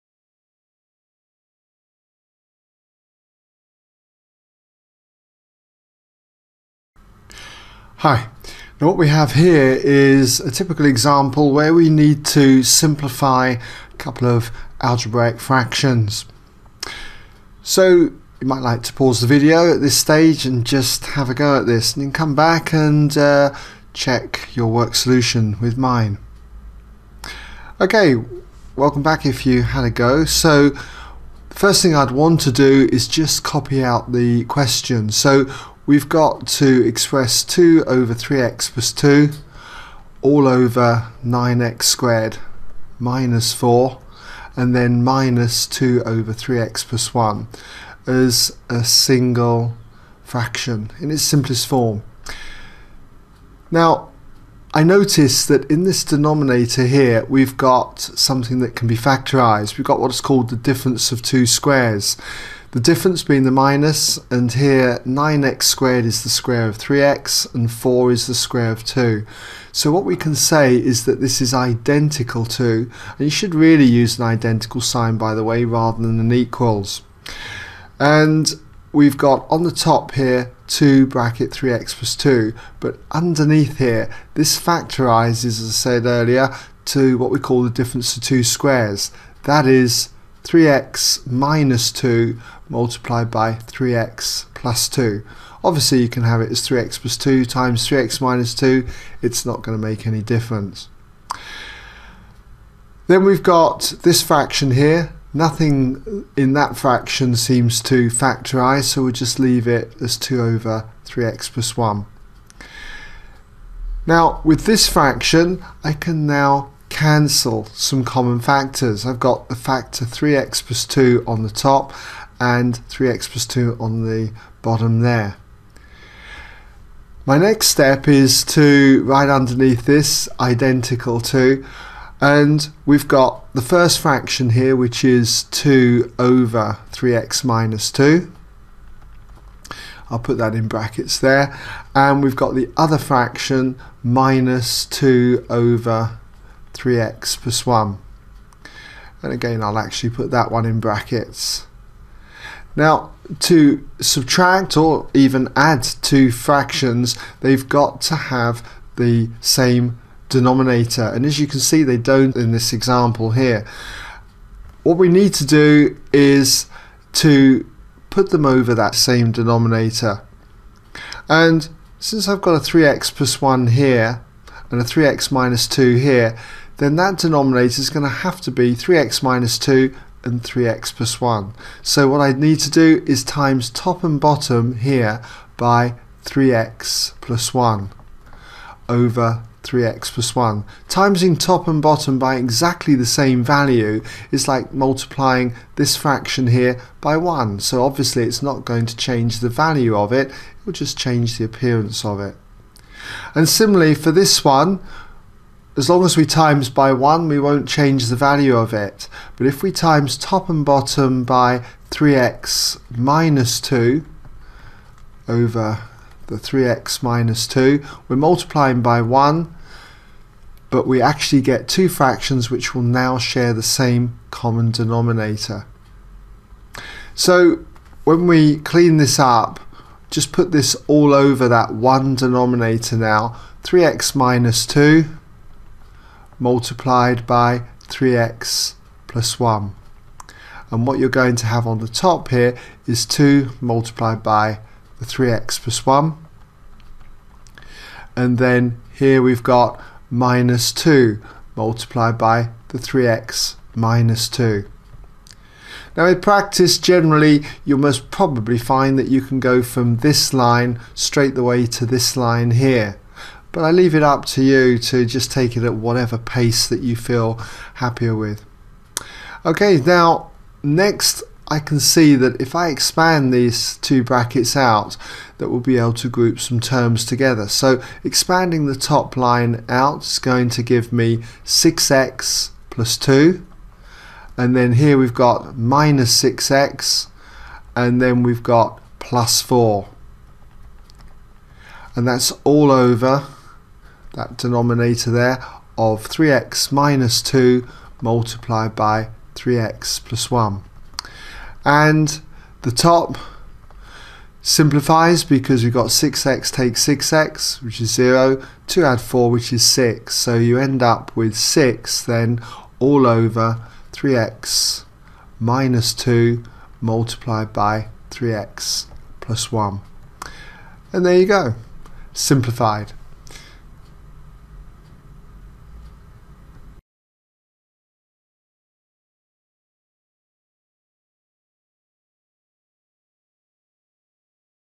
Rational Expressions - Addition, Subtraction | C3 Edexcel June 2012 Q1 | ExamSolutions
An example on simplifying rational expressions by adding together two algebraic fractions.